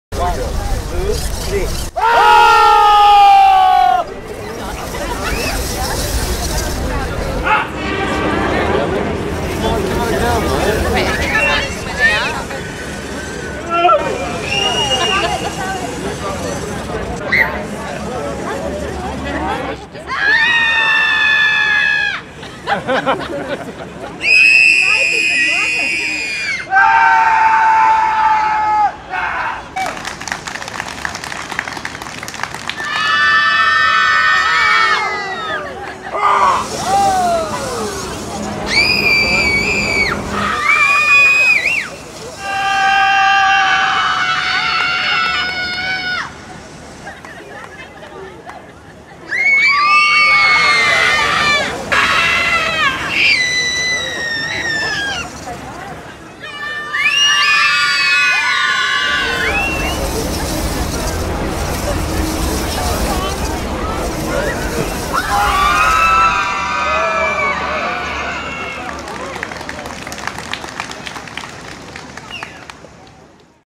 Sscreaming Rapture at Vivid Sydney (2012)
Interactive light and sound installation at Vivid Sydney

FRANK MAGUIRE
LIAM RYAN
JASON MCDERMOTT

EDWIN BUDHI